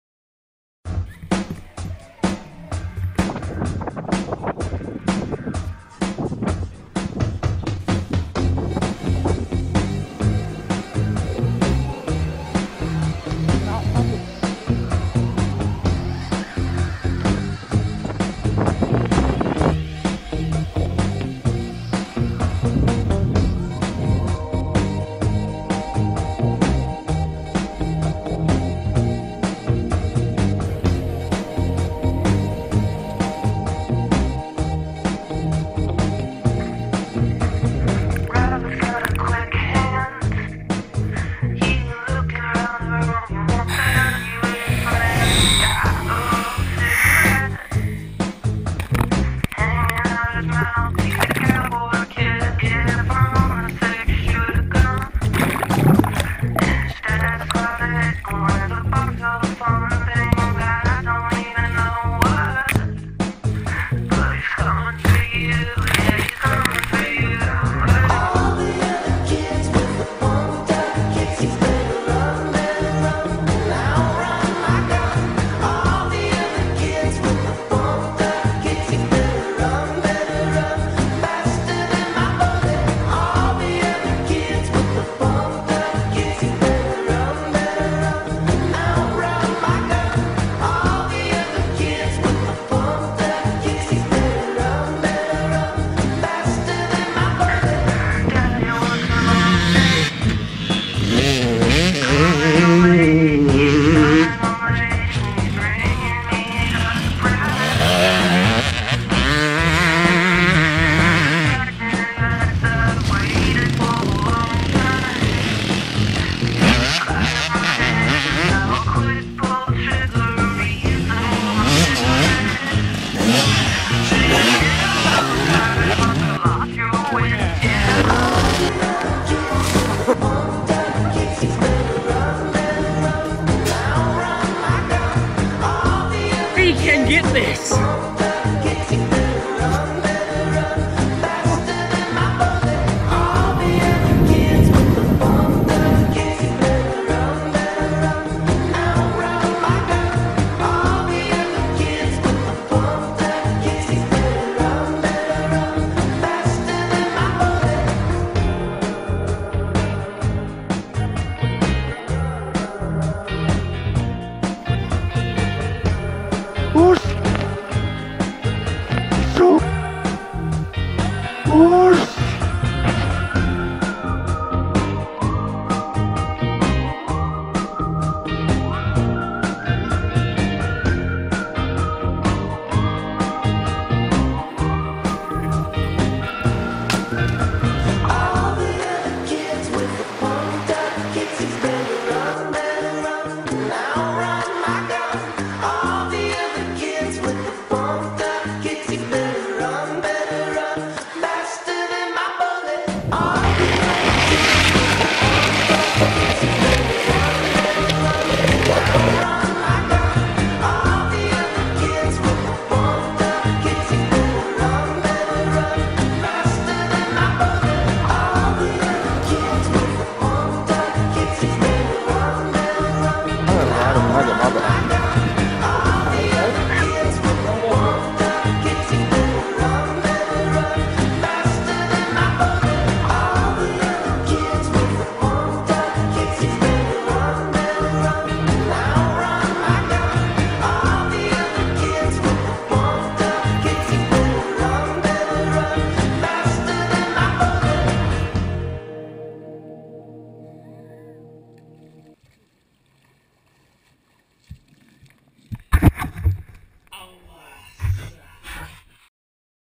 Aidan Quinn (video pad)
just a movie of cleaning up my i pod of some of the cool stuff me and aidan did. This is like not even half though